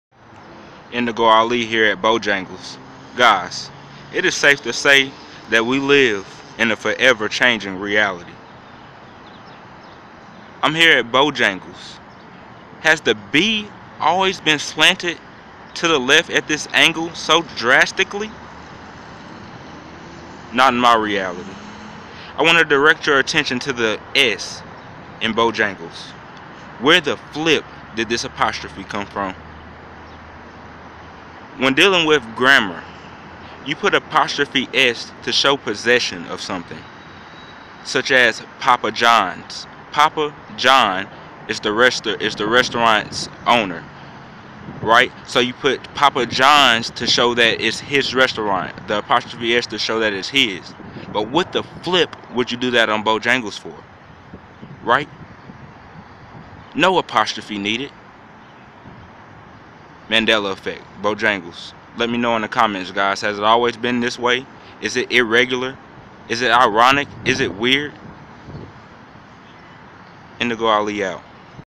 Mandela Effect Bojangles
The 'B' is practically laying down sleep and there is a possessive apostrophe above the 's' huge mandela effects